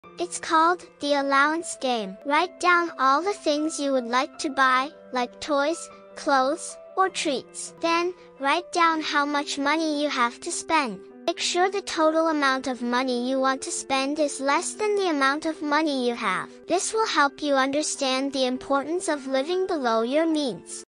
Join the Allowance Game with Kora: Learn to Control Spending!
Get ready to play the Allowance Game with Kora, your AI financial guide! This exciting activity, inspired by financial literacy books, is designed to teach kids about controlling spending and managing their allowance effectively. As future financial experts, this game will help you understand the value of money and make wise spending decisions. Let's play and learn together!